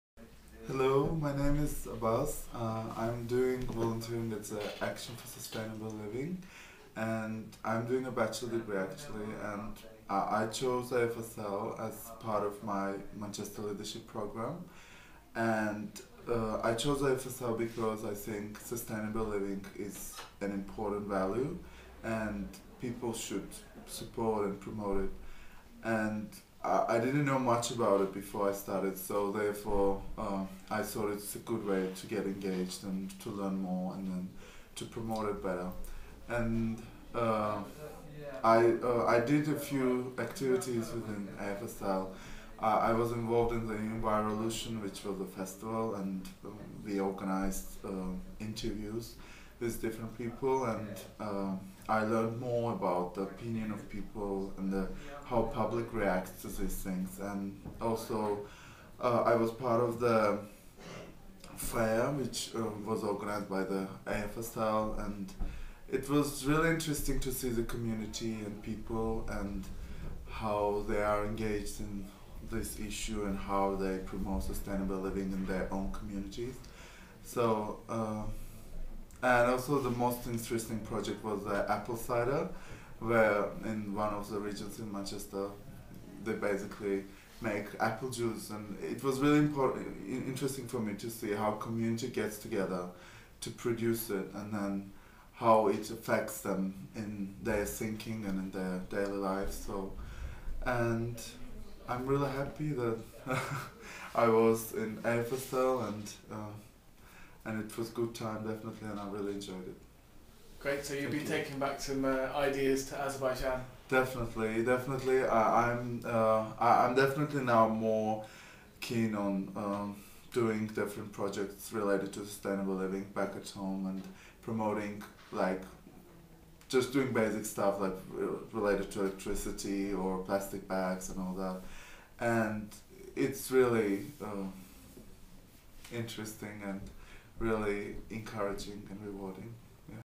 Abbas Abbasov, Community Volunteer for Action for Sustainable Living projects
Abbas volunteered with us while on an exchange program to Manchester University, through the univesitie's Manchester Leadership Program (MLP). Though only in the UK for a short time, he has been able to use his free time to best advantage and gained some really useful experience.